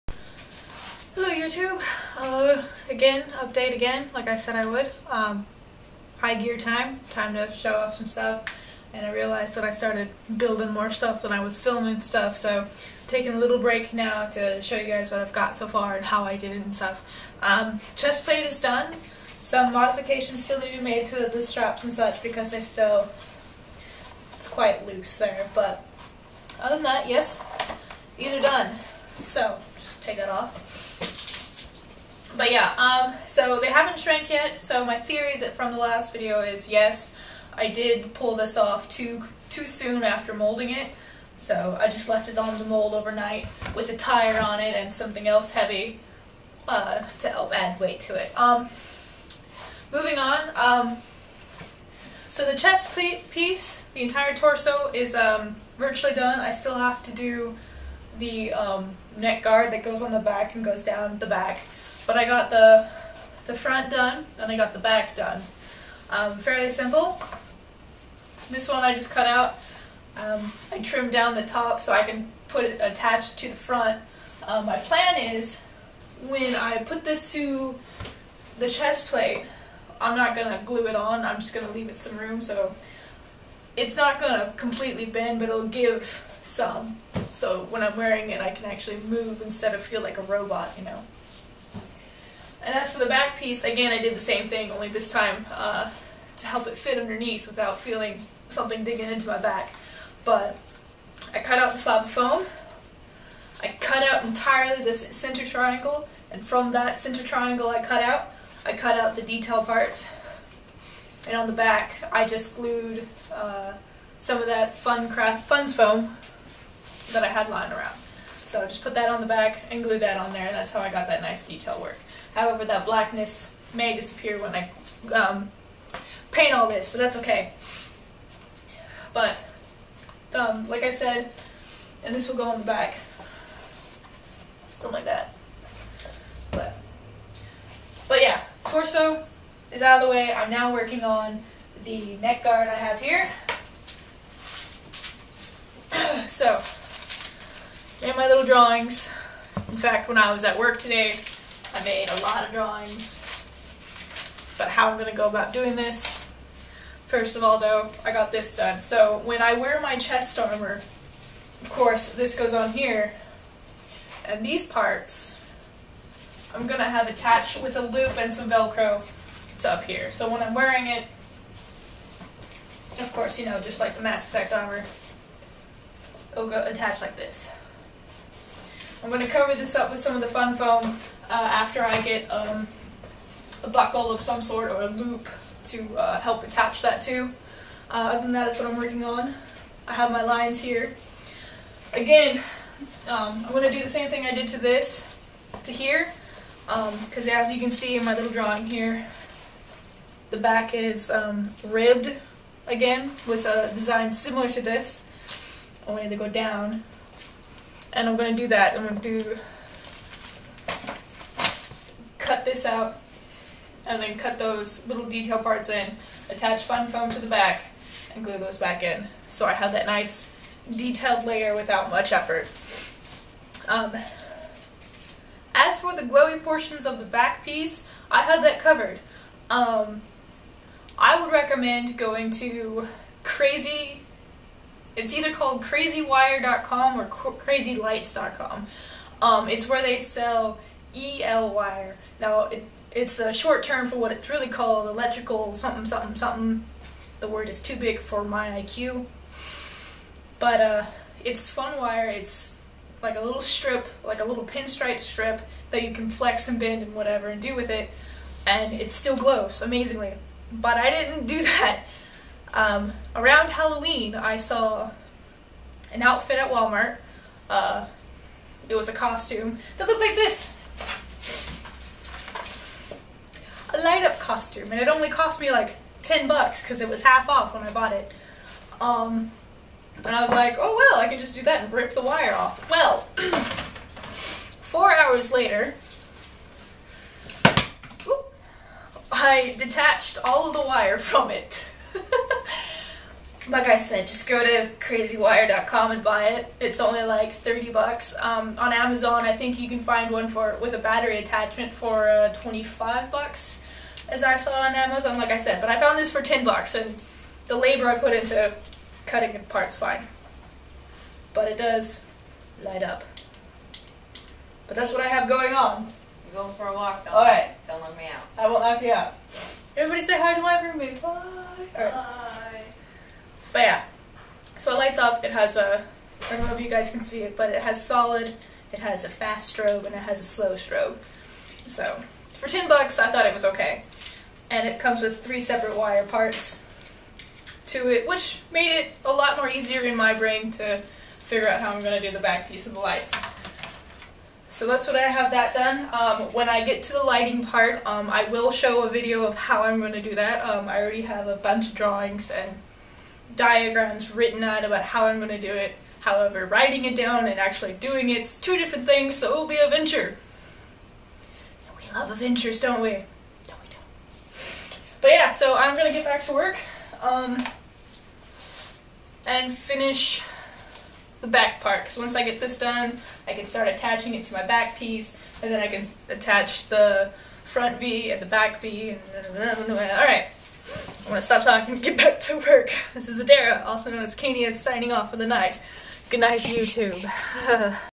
Mass effect cosplay build: Chest plate completed (mostly)
Done with the front and back parts of the chest pieces. Now on to the the neck piece and shoulders!

As for the EL wire that I had mention, I am sorry that I had my information of where to get EL wire online was wrong. EL wire is sadly only sold online (or so the internet tells me) but you can purchase the stuff from either of these two sites and/or from Amazon.com.